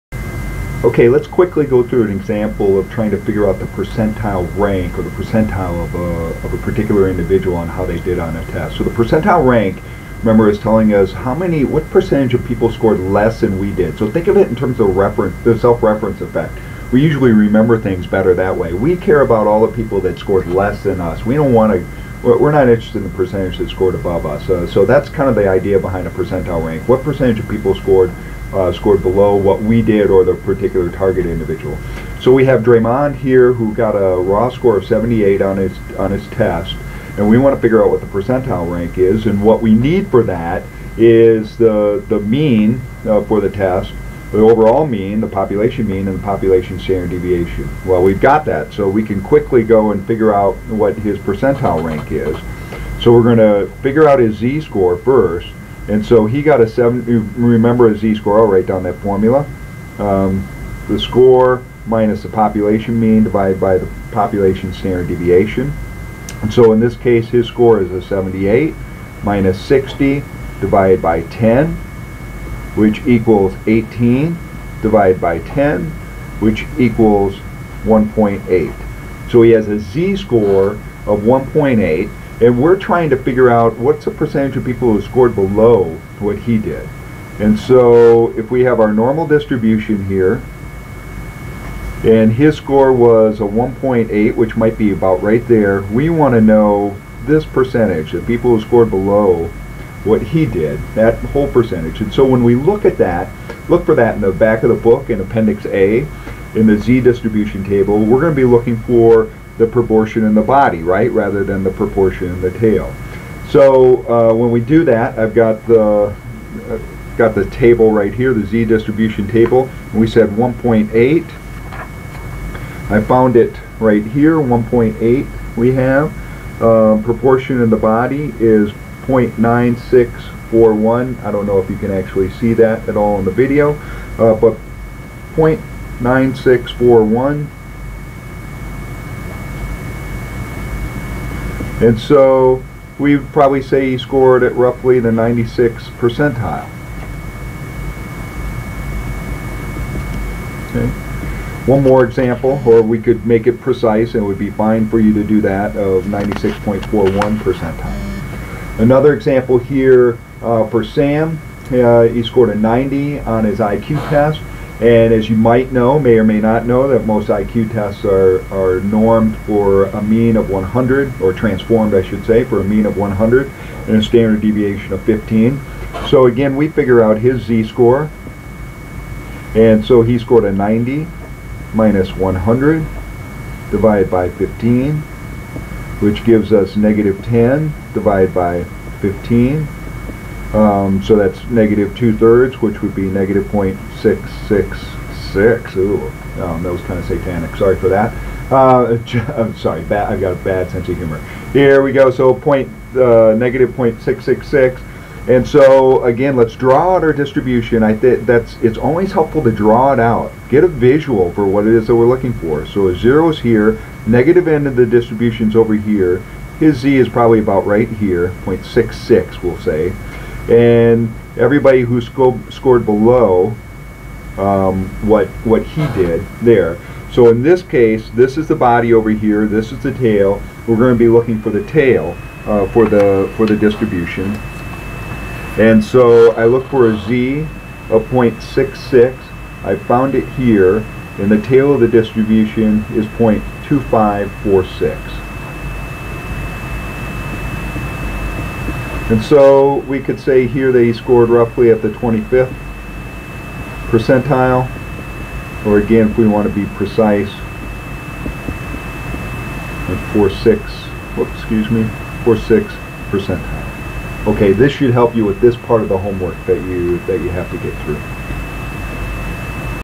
Percentile Rank
Video for PSYC335M Intro to Statistics. This video will help you with the homework problem regarding figuring out a person's percentile rank.